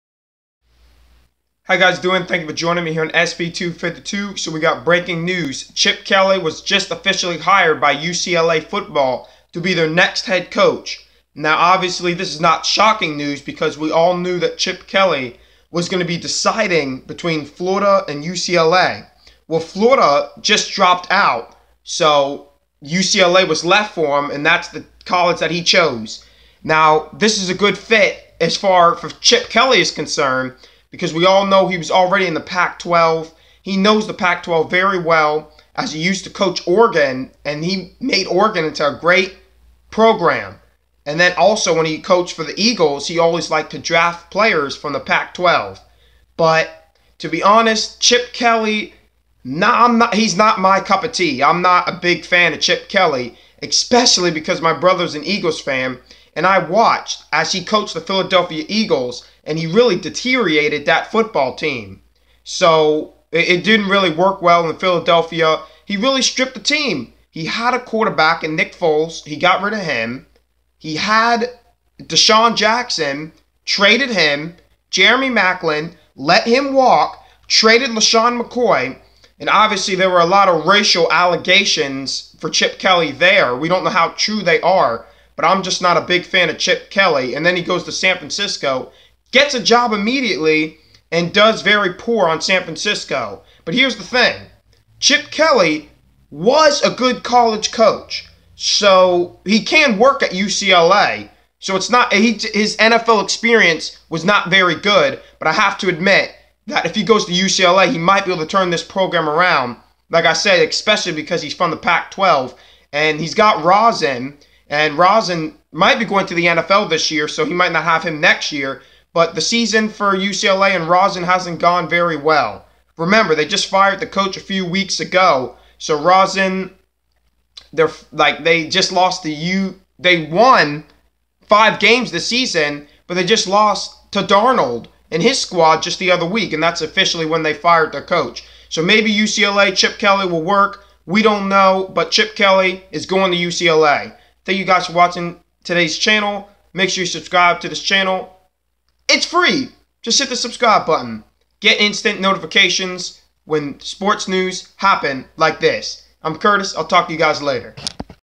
Chip Kelly hired as UCLA Head Coach
Former nfl head coach Chip Kelly has found another College Job!